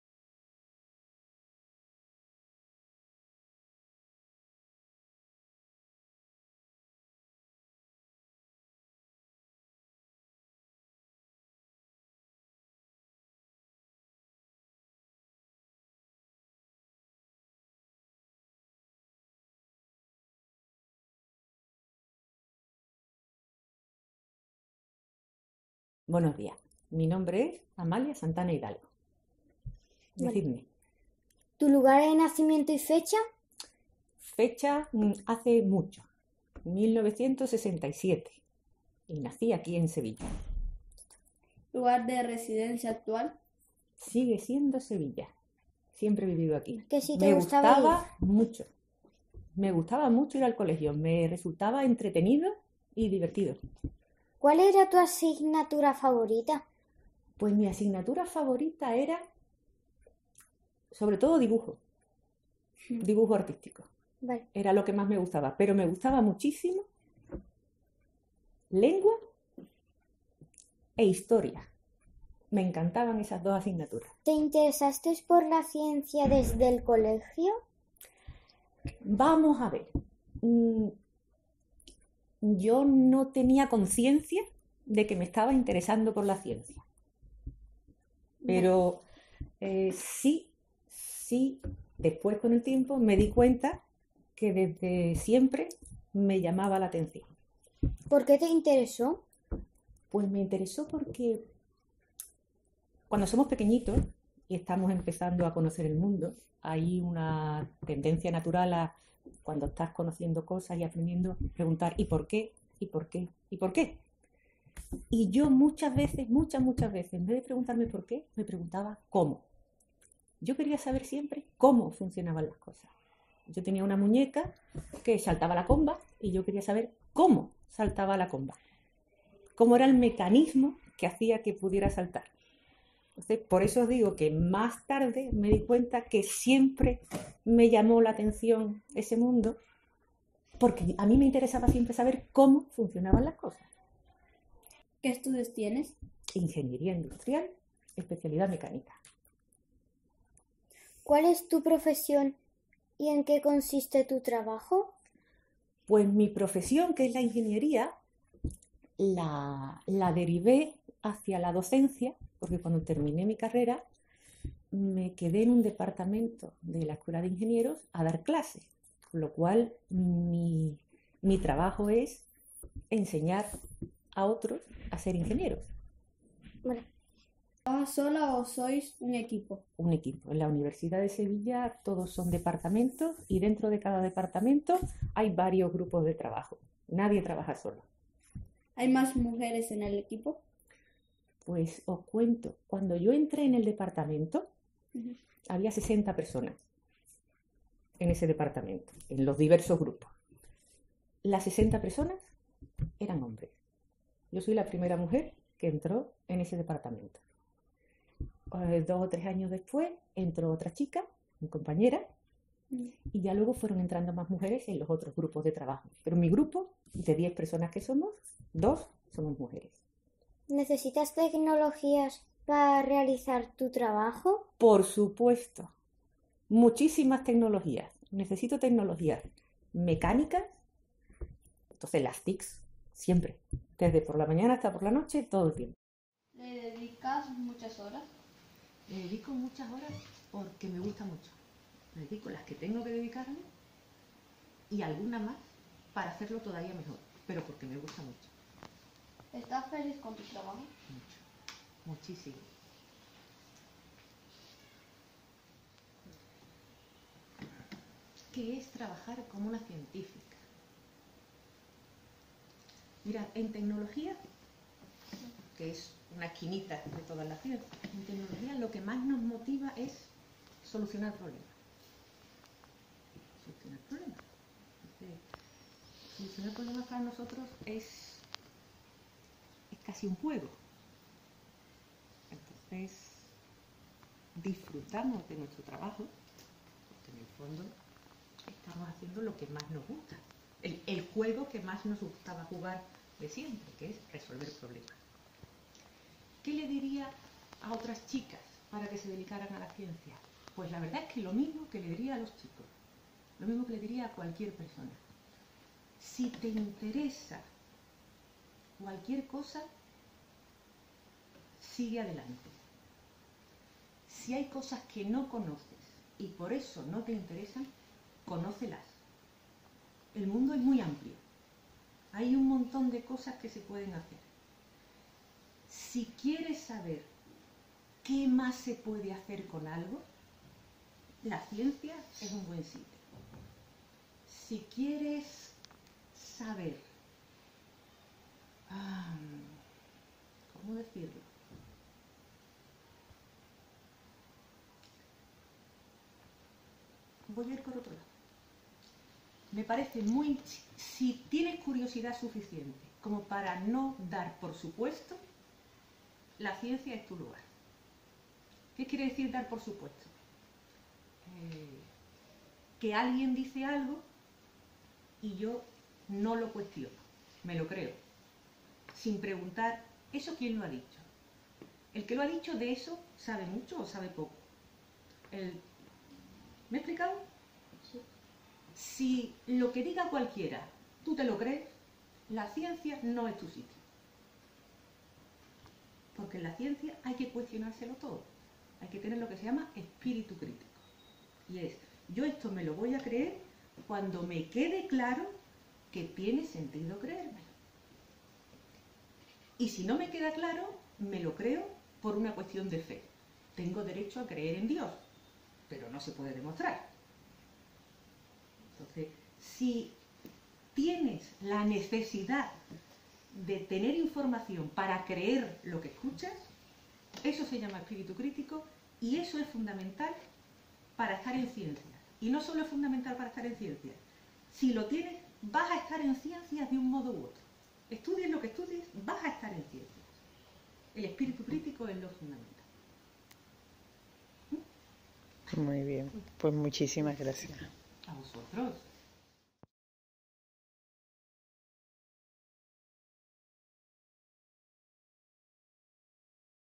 Entrevista a Amalia Santana Hidalgo
Colegio Huerta Del Carmen de Sevilla
Entrevista a Ana Lorente Barragán Realizada por Daniel y Crishell
Creado para el concurso Wisibilizalas organizado por la UPF la Universidad Pompeu Fabra de Barcelona.
Gracias Amalia por colaborar con nosotras.